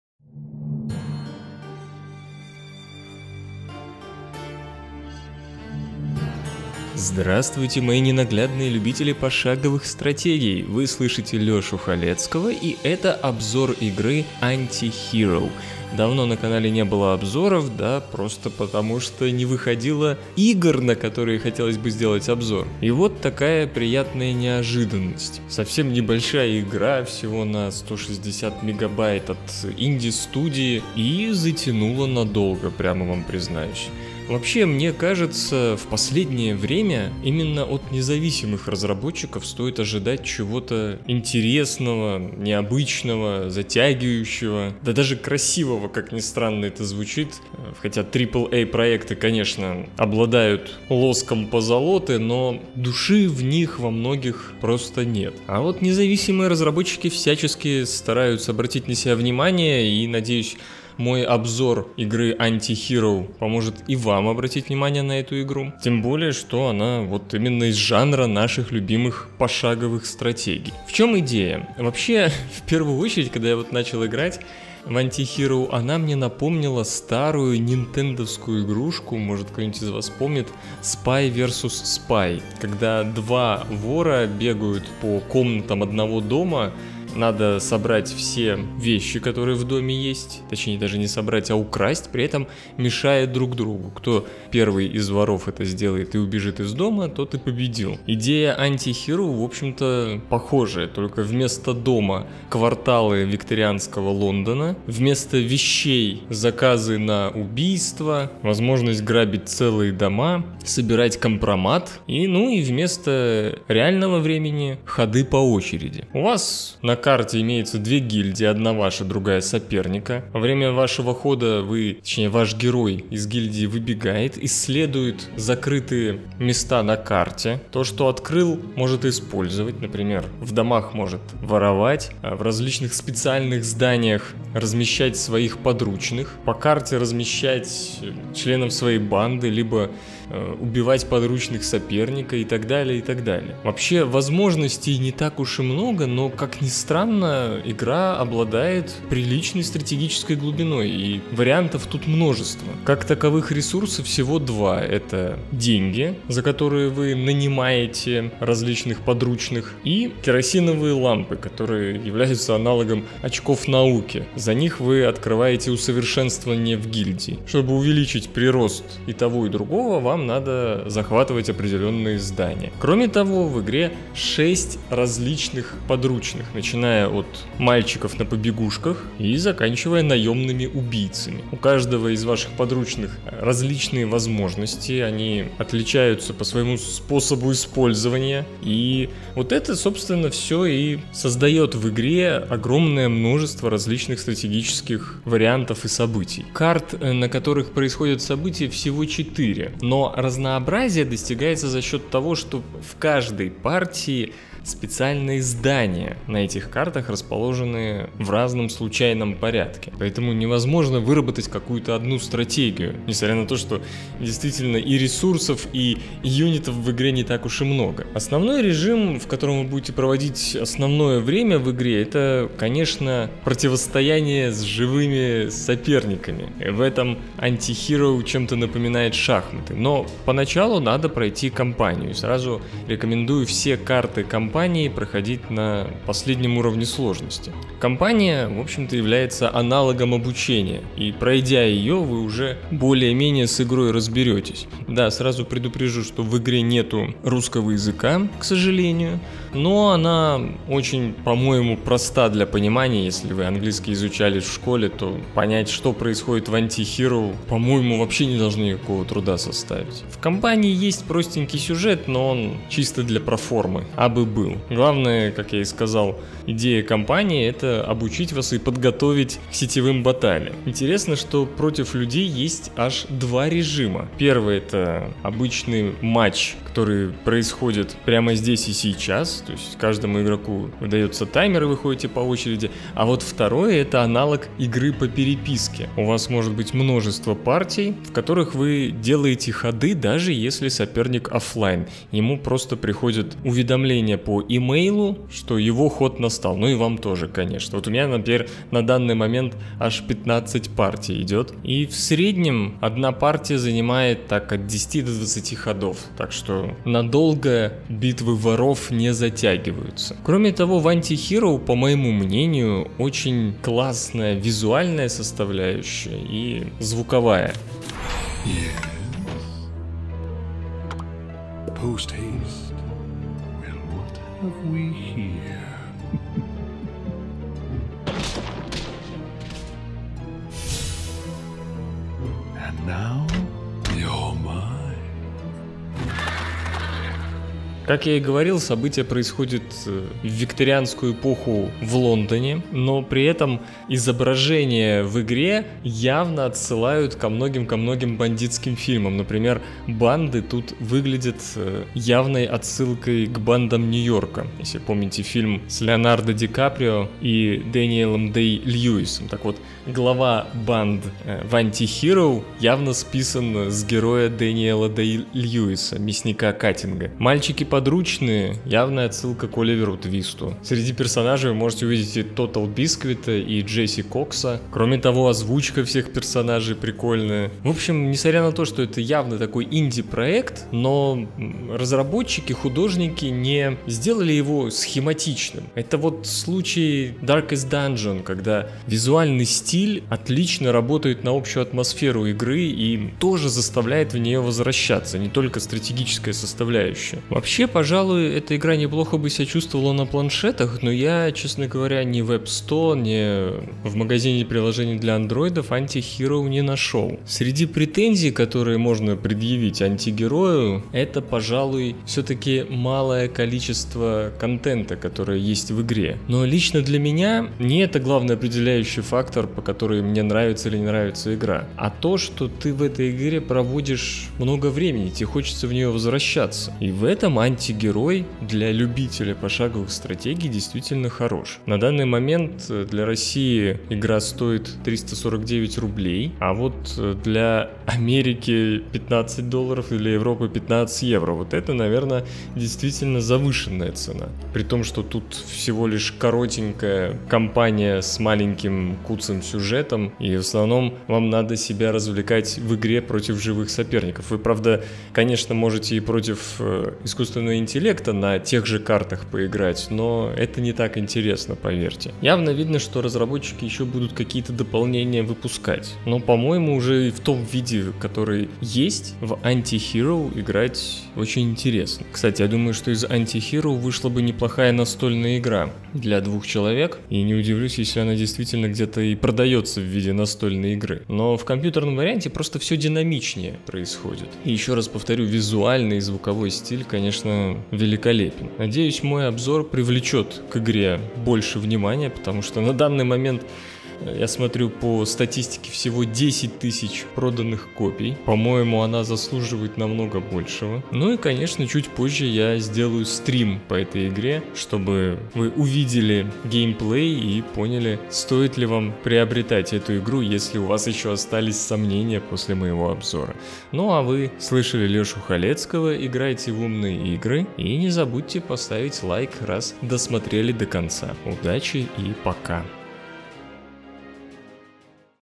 Antihero: обзор игры и рецензия
Два вора в пошаговом режиме борются друг с другом в кварталах викторианского Лондона. В ход идёт всё: шантаж, грабёжи, убийства. Описание интригует? Так ещё и игра не так проста как кажется! Смотрите мой обзор.

Не забывайте делиться моими видео, лайкать их и подписываться на канал!